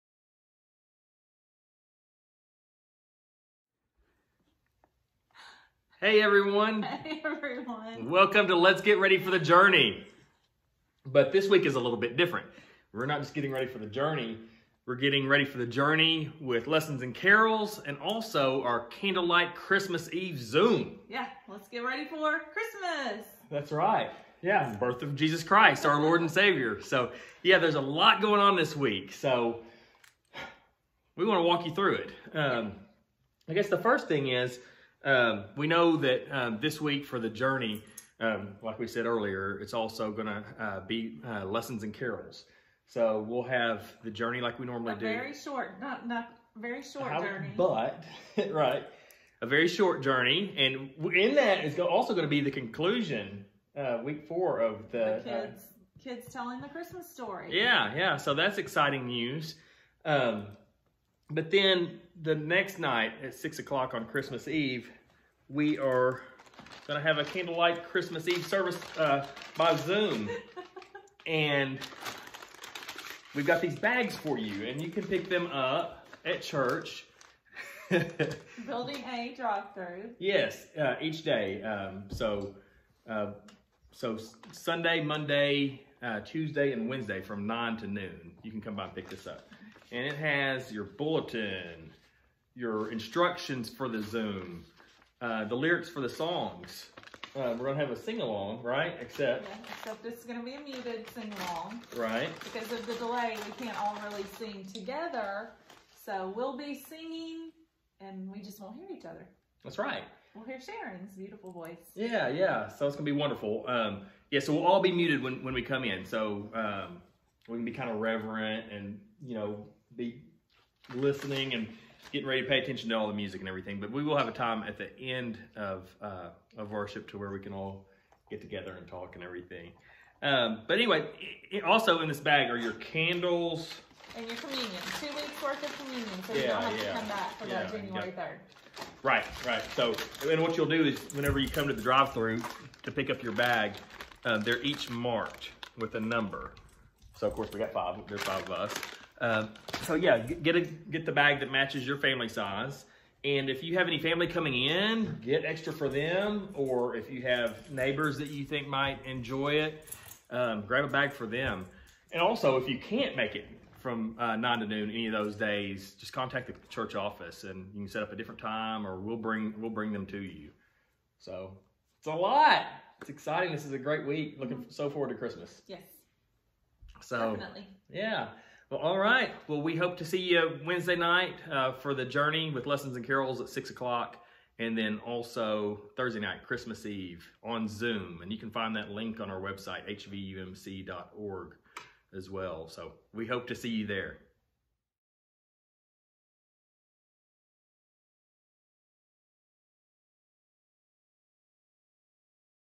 Let's get ready for The JOURNEY Christmas Week
Join us Christmas week for The JOURNEY with Lessons & Carols Wednesday night at 6:00 on Facebook and YouTube and our Christmas Eve Candlelight service Thursday night at 6:00 on Zoom.